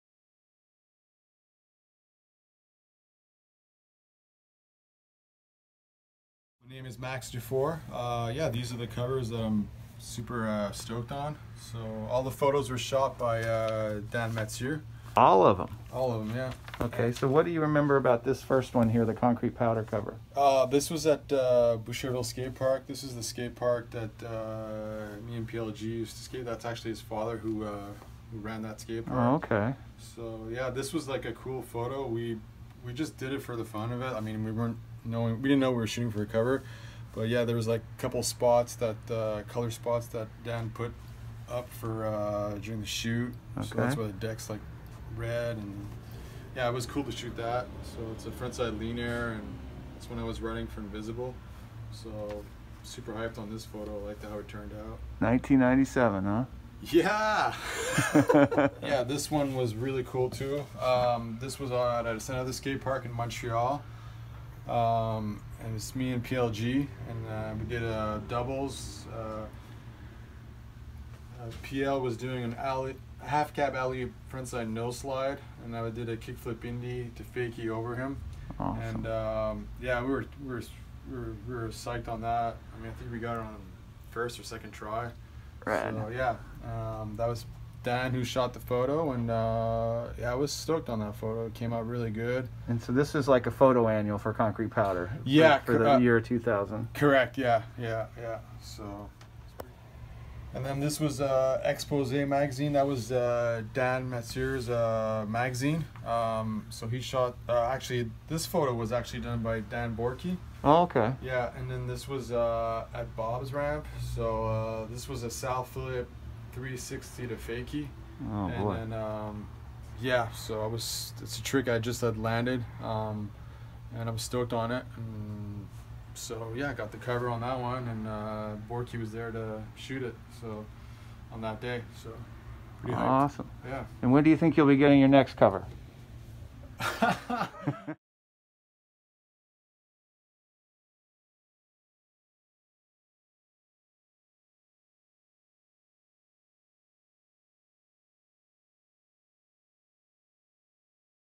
cover discussions - Max Dufour
Max tells about his Concrete Powder cover and his Concrete Photo Annual cover both shot by Daniel Mathieu and ends it with his Expose cover shot by Dan Bourqui at Bob Burnquist's ramp... shot on location in Oceanside January 2021